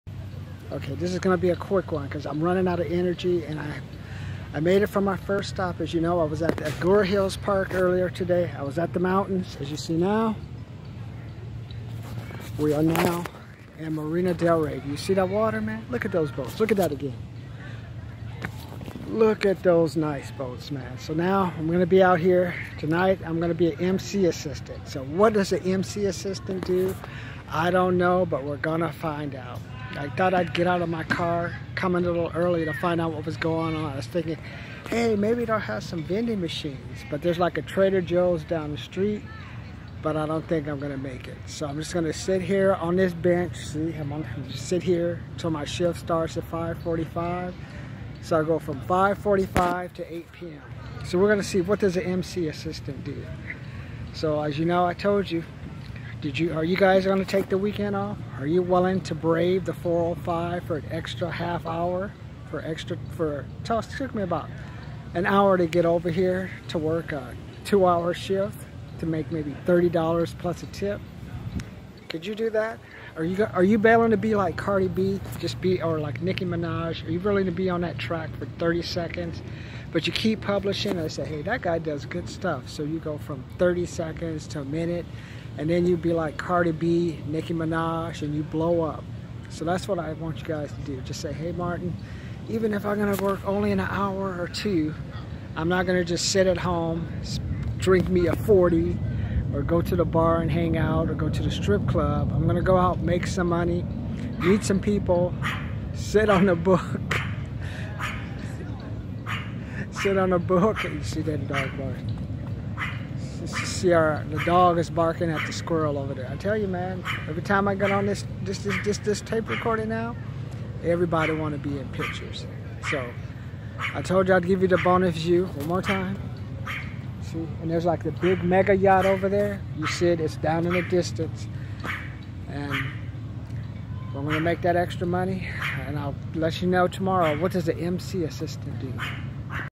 At the Marina in Marina Del Rey
Starting my second shift at the Marina as a Mc assistant What does an Mc assistant do am I able to get on the miceee and spit a rhyme  My name is Martin I here tonight to spin you some lines for you to hear and remember the rest of year to listen to what you hear at the Marina so you do mess up your money and have to eat Purina!!!